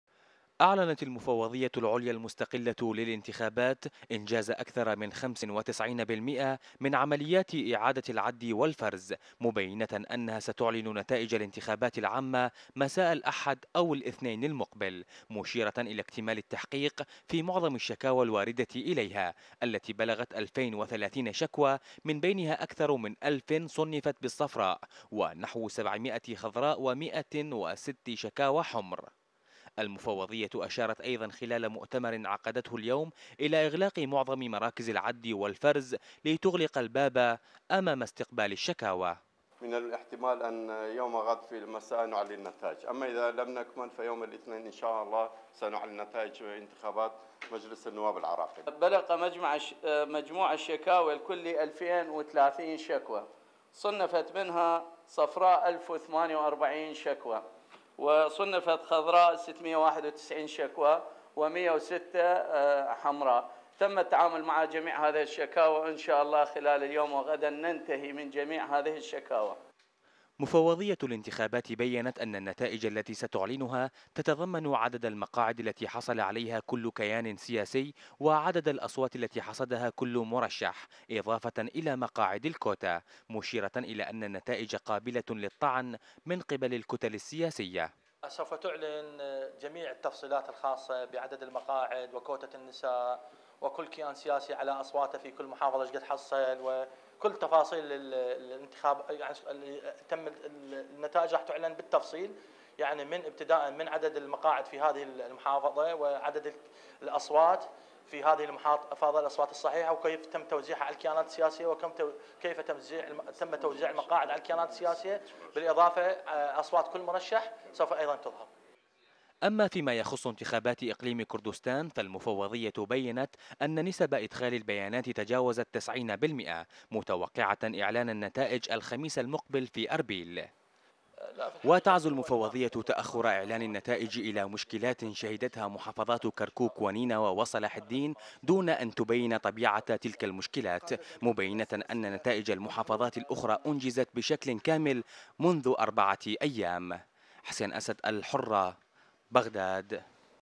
مفوضية الانتخابات تكشف عن نسب المشاركة في الانتخابات وتعلن موعد النتائج الاولية للانتخابات 5/17
مفوضية الانتخابات تكشف عن نسب المشاركة في الانتخابات وتعلن موعد النتائج الاولية للانتخابات
حسين اسد